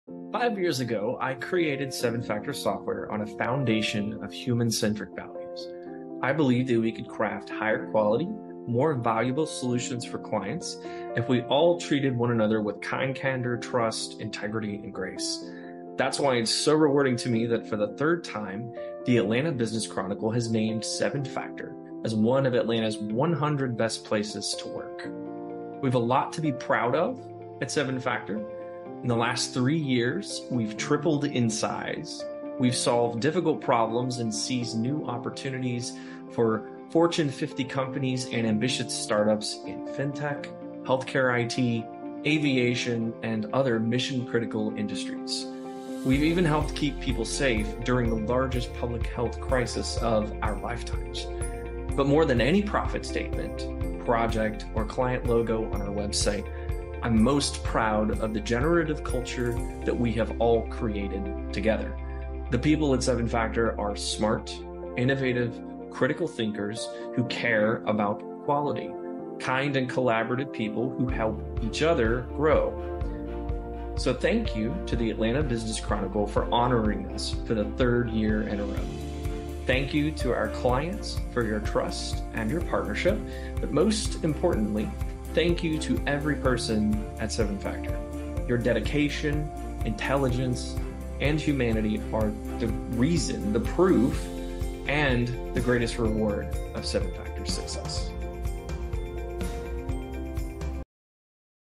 7Factor Software is a Best Place to Work
7Factor Software has been deemed a Best Place to Work by the Atlanta Business Chronicle for the THIRD year in a row! This achievement reinstates that our dedication to creating a human-centric culture inspires our people to create the best solutions.